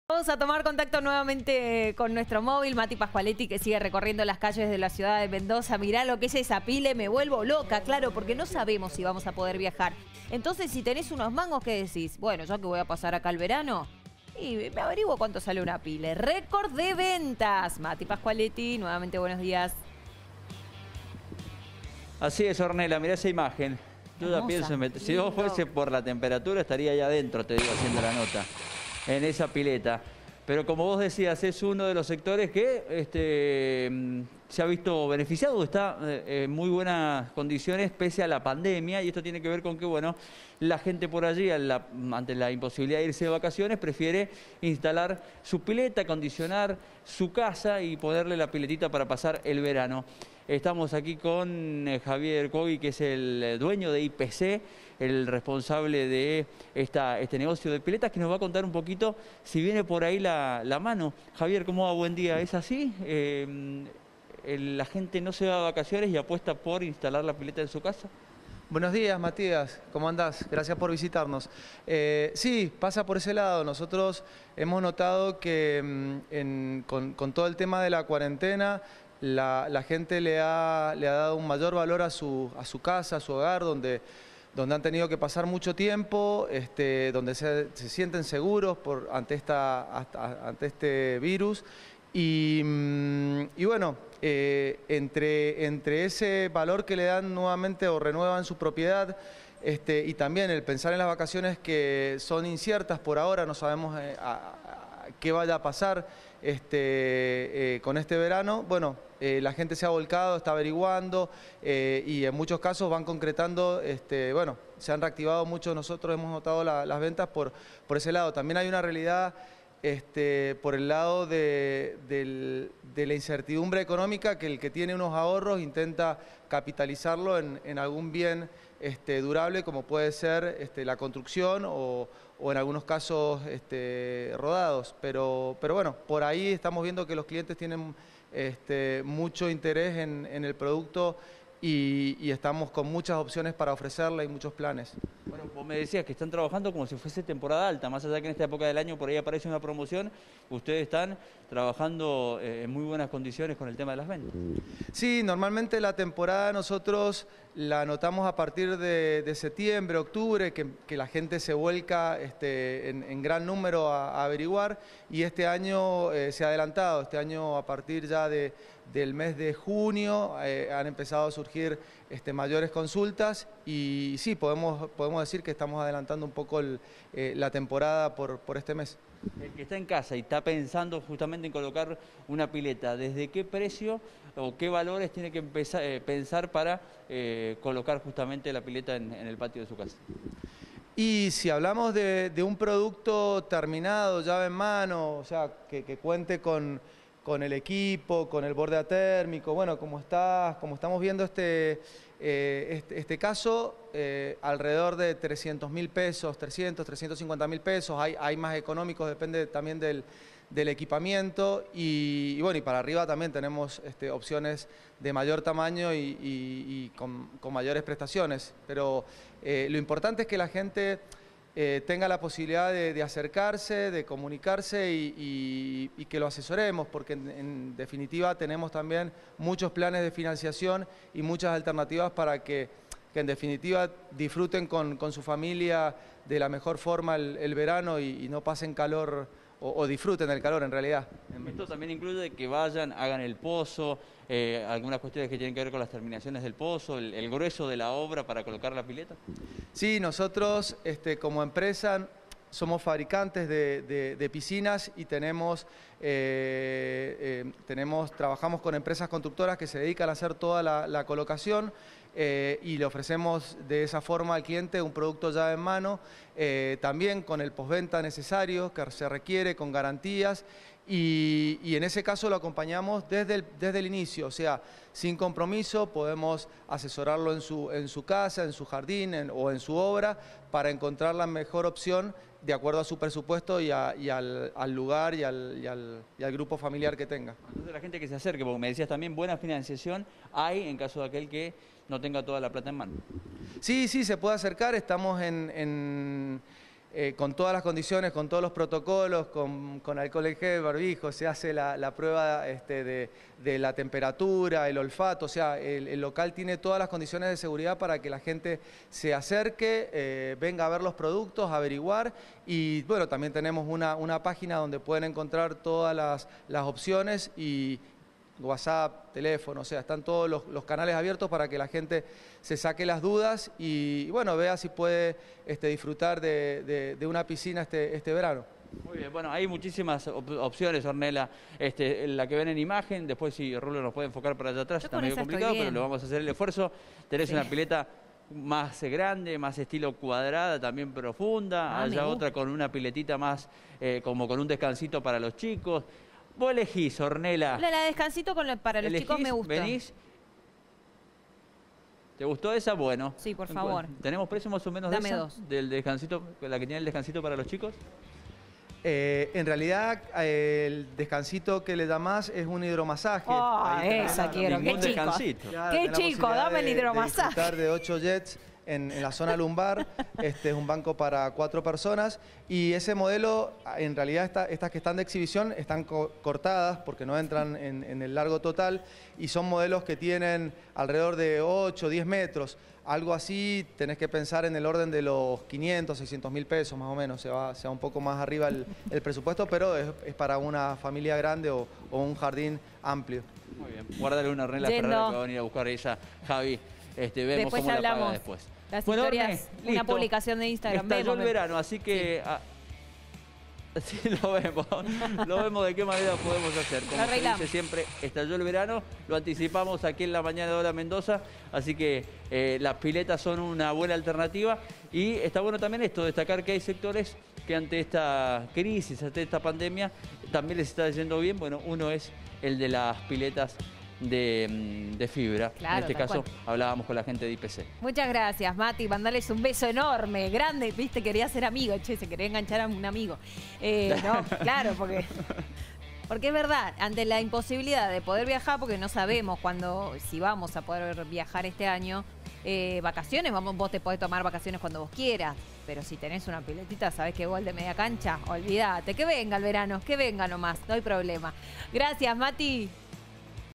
INCREMENTO EN LA VENTAS DE PILETAS
Creció la venta de piletas.
 Ante la posibilidad de quedarnos sin vacaciones.